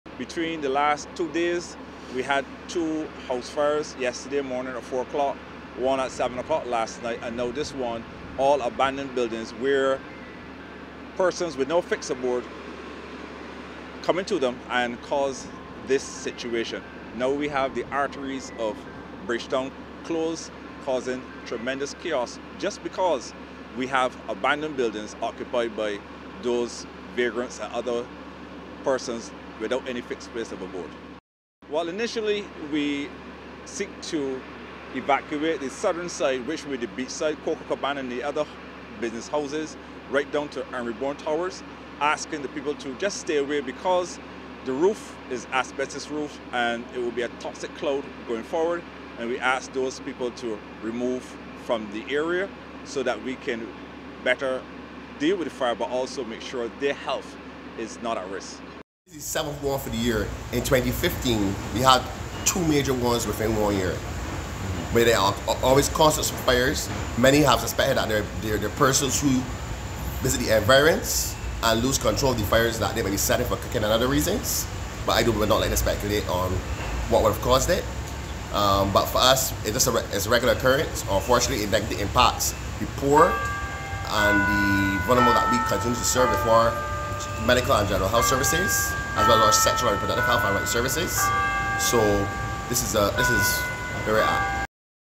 Fires in abandoned buildings a concern
Fire and rain in the City made for a toxic and uncomfortable mix for many Barbadians this morning.

A passerby alerted staff at the Barbados Family Planning Association (BFPA) that smoke was billowing from an adjacent building on Jemmotts Lane which contained asbestos.

The smoke forced the evacuation of people from nearby buildings and businesses.

According to reports, the Fire Service arrived around twenty to eleven and started to out the flames which were contained in a section of the deserted block.

Of major concern to the fire officers is the frequency of fires at abandoned buildings.

Meanwhile, Executive Director of the BFPA Anderson Langdon shares the concerns of Station Officer Bourne regarding fires at abandoned structures.

Mr. Langdon says given the number of occurrences they have been in discussion with the relevant ministries and are in the process of looking to relocate, but they need funding to complete the process.